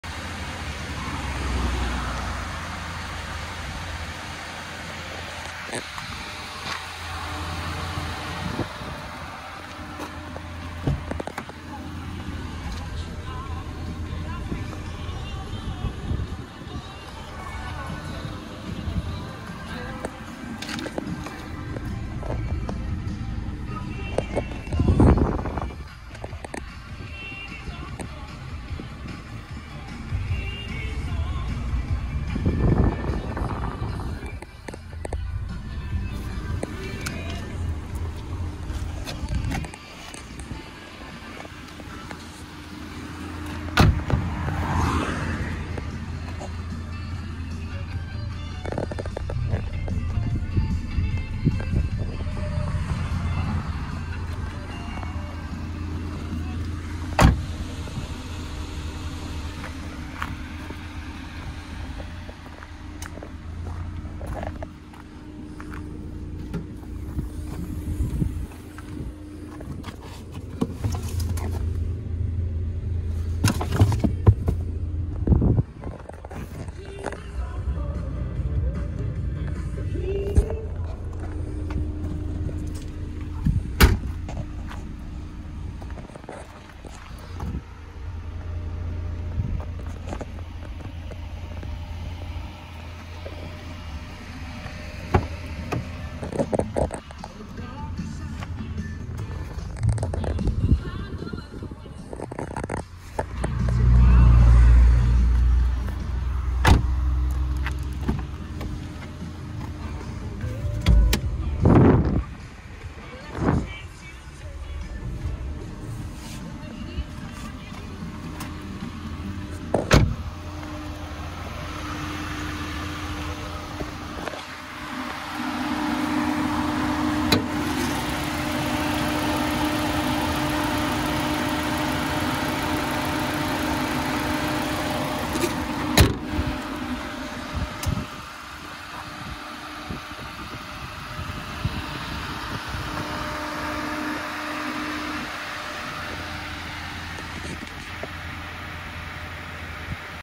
AK10 CMU Ford Fusion 1.4 Style + Only 36,000 Miles Full History 13 Stamps
Amazing Little Fusion With Only 36,000 Miles ,
2010 (10) Ford Fusion 1.4 Style + 5  dr,
Full  Service History 13 Service  Stamps Just Serviced,
Only 36,000 Miles,
12 Months MOT at time of sale,
Specifications Include:,
Remote Central Locking 2 Keys,
Rear Park Assist,
Air Conditioning,
Radio CD Player,
Rear Load Cover,
Electric Windows,
Wheels Thick & Chunky Tyres,
ABS Braking,
To List But A Few,
More Photos on Website!,
Come on down and test drive TODAY!!,
Part Exchange Available WHY??,